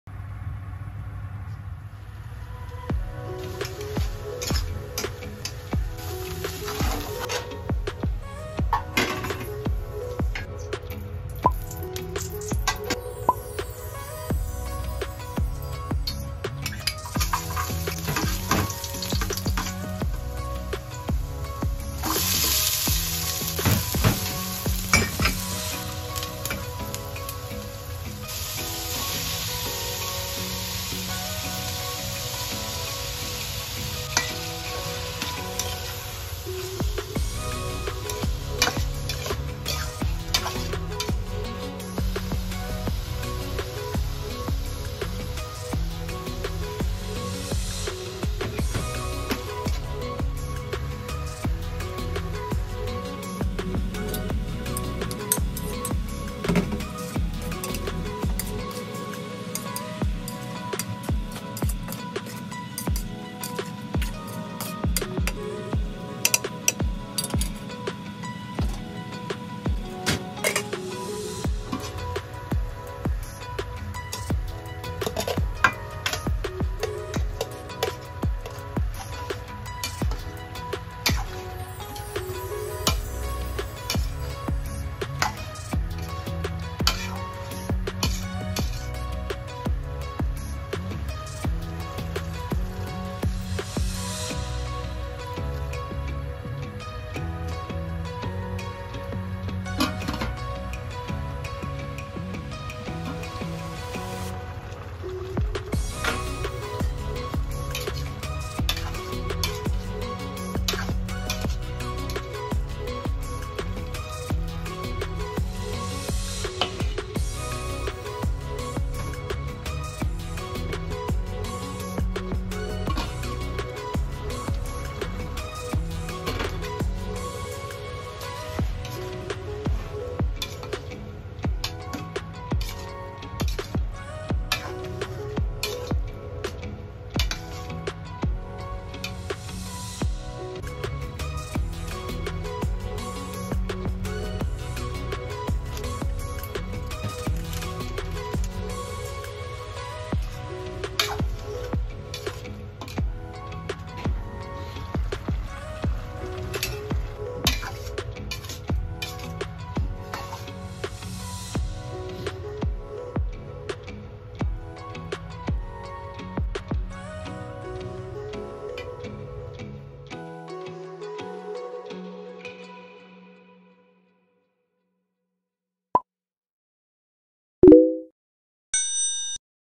Kilayin kapampangan / lutong kapampangan
Mgakakailanganin at sangkap

1pack bopis set

Bawang
Sibuyas
Toyo
Suka
Dahon ng laurel
Paminta
Dried sili
2 cups tubig